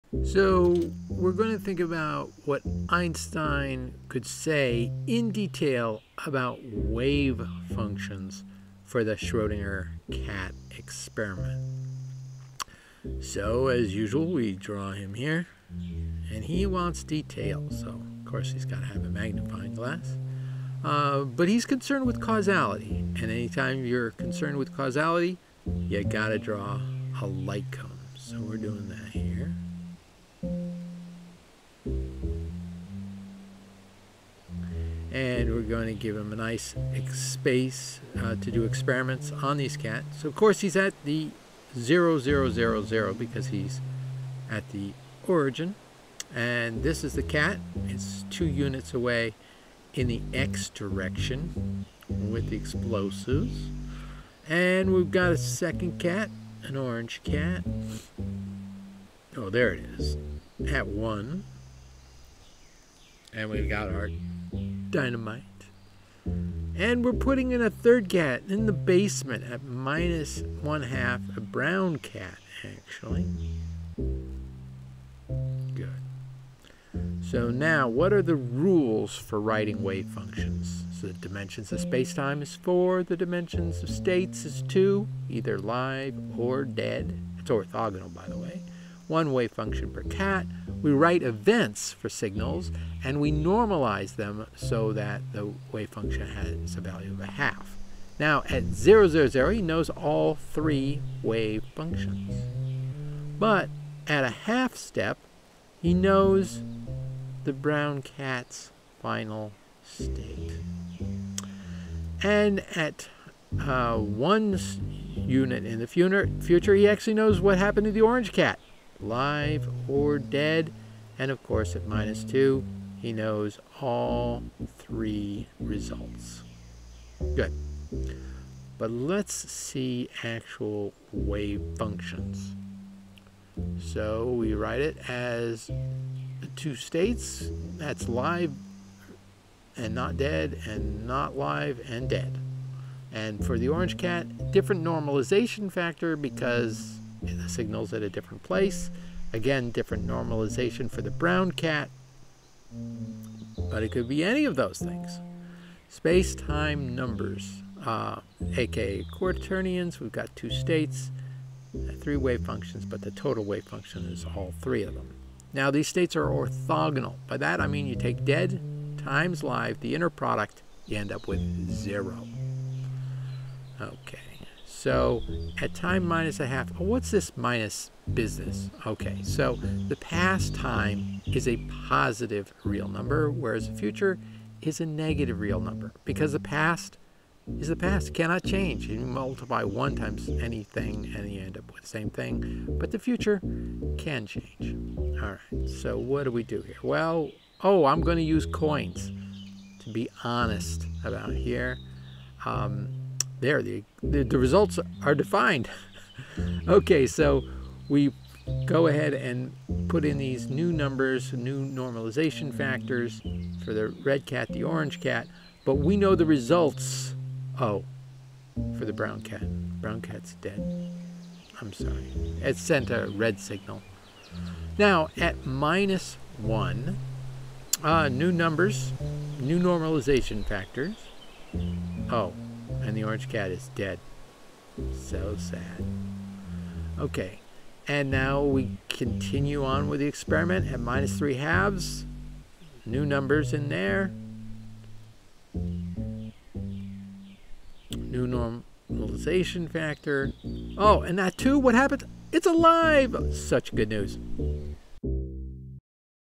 Understanding Schrödingers cats, Part 2 of 2, The Wave Function
In 2011, Henry Reich posted a great video on the Schrödinger's cat experiment, available This is my effort to explain the thought experiment. Space-time numbers (aka quaternions) are used instead of complex numbers because space-time numbers live at obvious places in a space-time diagram. Einstein gets to use a light cone which is needed to understand causality. The events that are in the wave function are space-like separated from the observer (Einstein in this case).

In Part 2, the wave function is drawn at different moments in space-time: the start, then each half step afterward as Einstein eventually learns the fate of each of the three cats. The video was tried a few times. As a random process was used, one time all the cats lived, another they all died. You will have to watch to see what got uploaded here.